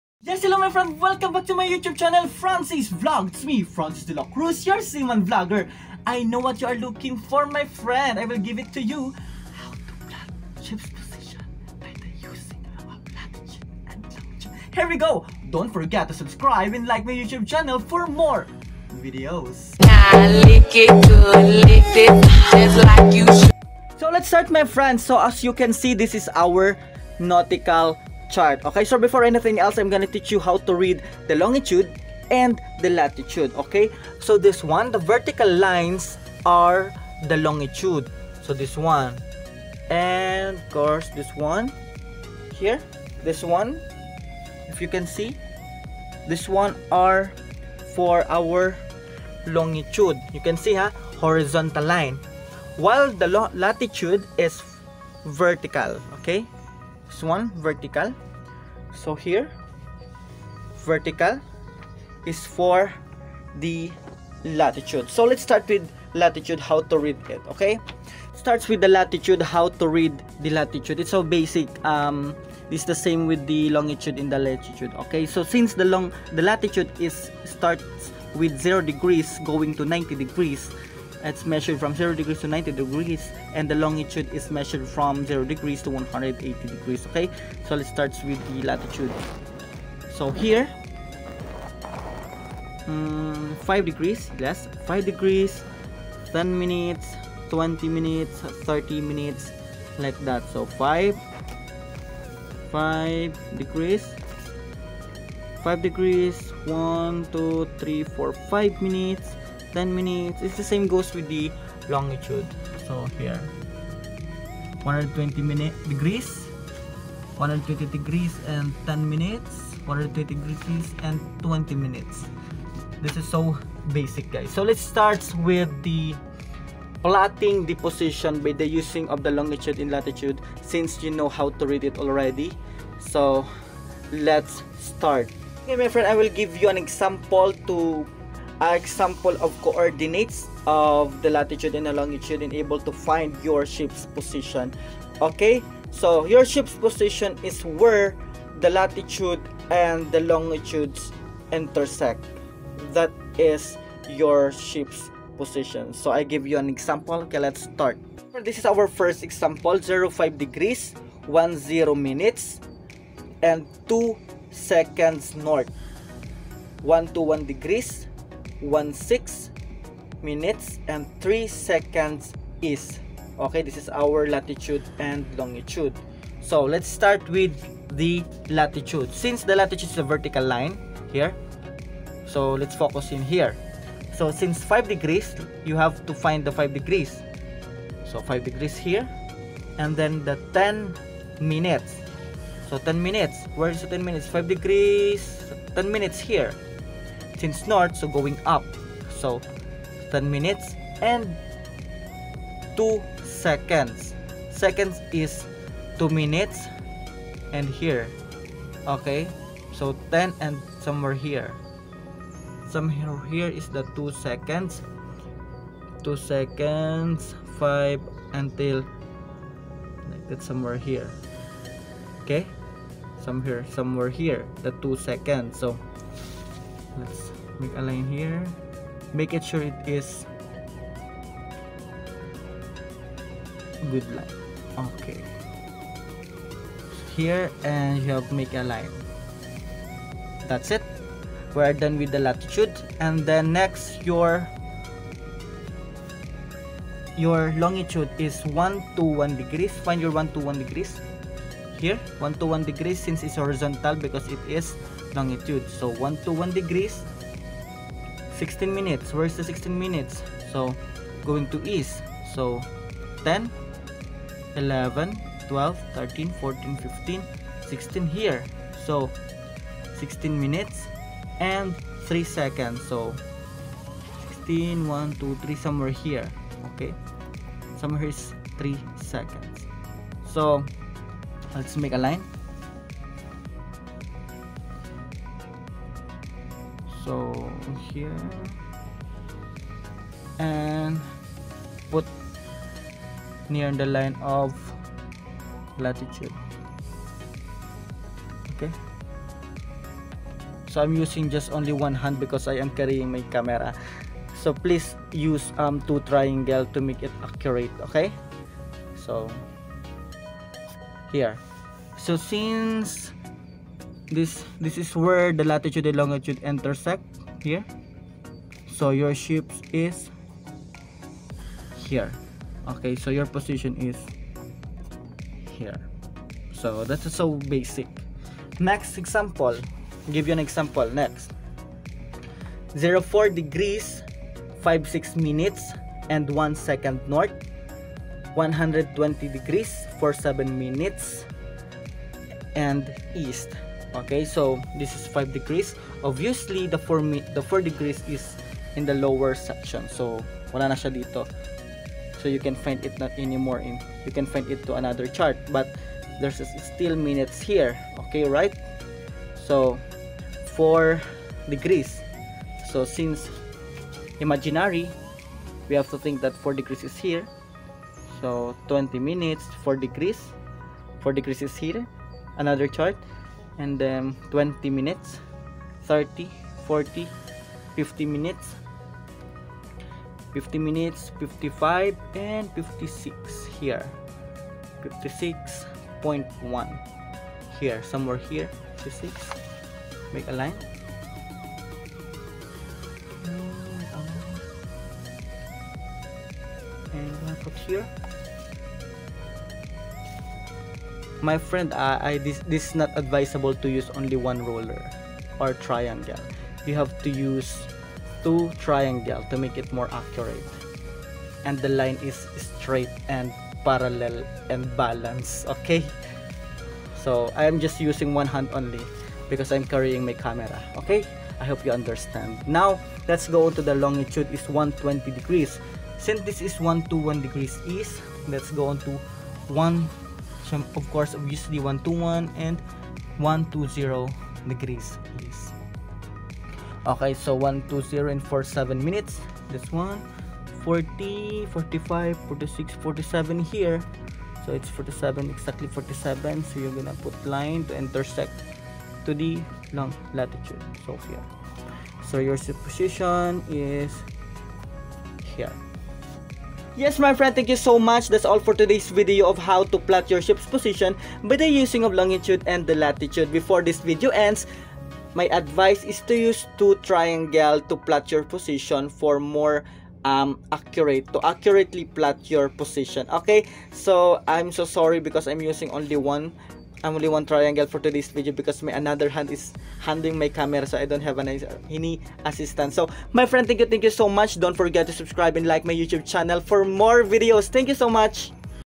HOW TO PLOT SHIPS POSITION ON A CHART USING LATITUDE AND LONGITUDE
It is important to monitor the ships position time to time. So this video will help you of how to plot ships position on a chart.